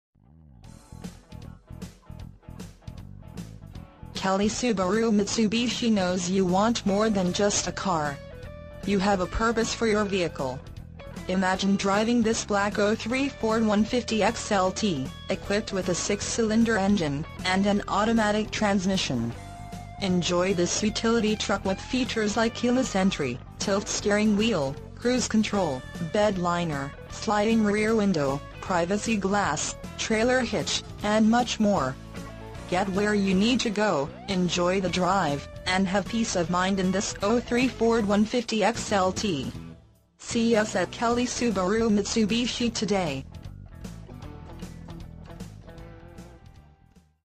SOLD - 2003 Ford F-150 XLT 37402 KELLY SUBARU MITSUBISHI
KELLY SUBARU MITSUBISHI

900 RIVERFRONT PARKWAY

2003 Ford F-150 XLT

VIN - 1FTRX17243NA51279

KELLY SUBARU MITSUBISHI knows you want more than just a car. You have a purpose for your vehicle. Imagine driving this Black 2003 Ford F-150 XLT, equipped with a 6 Cyl. engine  and an automatic transmission. Enjoy this utility truck with features like Keyless Entry, Tilt Steering Wheel, Cruise Control, Bed Liner, Sliding Rear Window, Privacy Glass, Trailer Hitch, and much more. Get where you need to go, enjoy the drive, and hav...